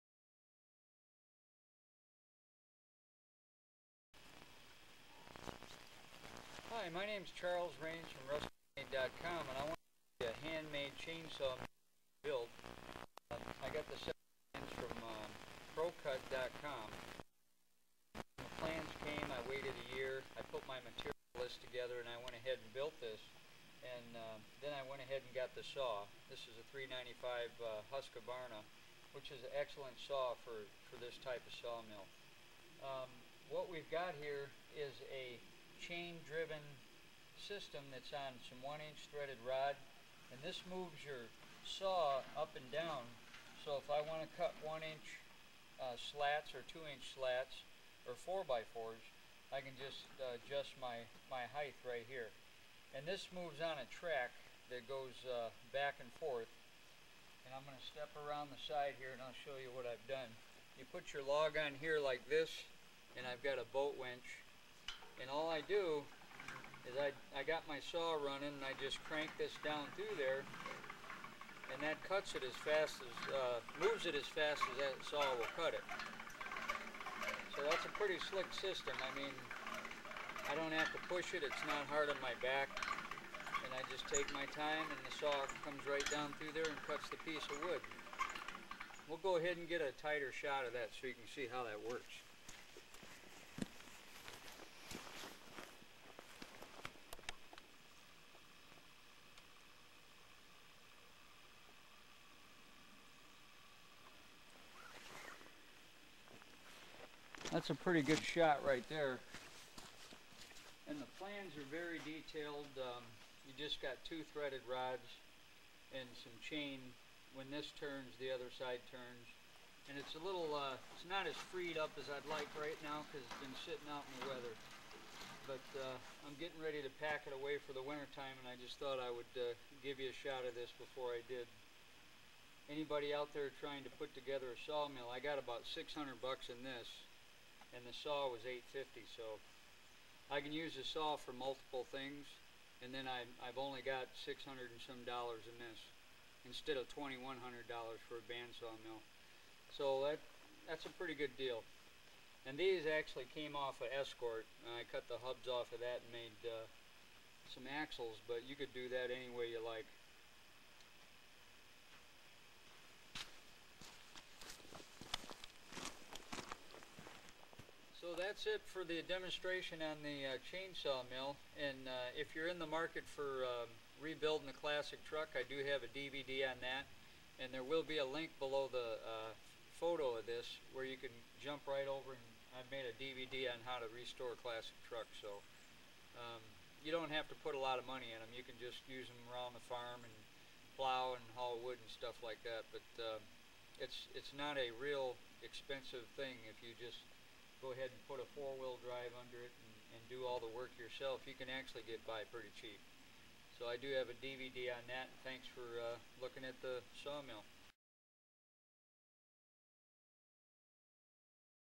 Build Your Own Chain Saw Mill
Demonstration of a home made chain saw mill